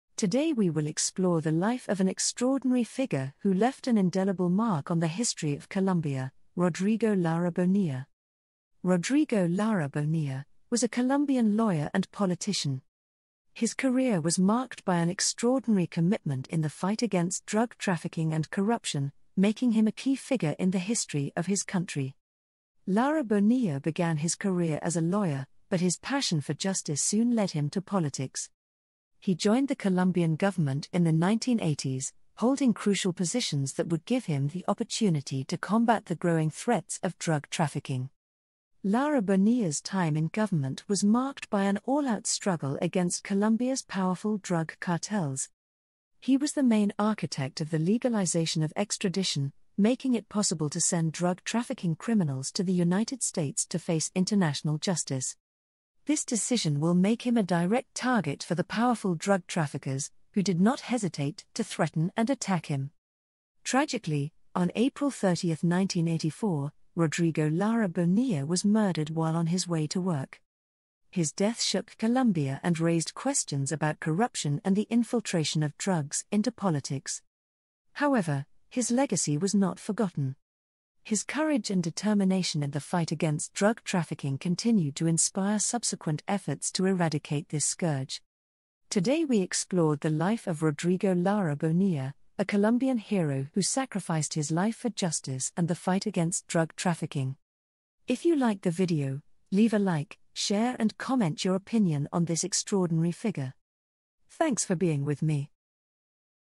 Rodrigo Lara Bonilla: The man who challenged Pablo Escobar
Today we will explore the extraordinary life of Rodrigo Lara Bonilla, a key figure in Colombian history. Through archive images, interviews and testimonies, we will shed light on the life of a man who fought for justice, transparency and democracy.

Rodrigo Lara Bonilla was a Colombian politician who courageously challenged the criminal power of drug trafficking during the 1980s. In the video, we follow his political career, examining his courageous initiatives against drug cartels and his fight for a country free from corruption.

We will also explore the tragic events that marked the premature end of his life and the lasting impact he had on Colombian politics. Rodrigo Lara Bonilla became a symbol of integrity and courage, inspiring subsequent generations to fight for a better future.

Sign up to stay updated on other content. Thank you for joining us in this tribute to one of Colombia's heroes.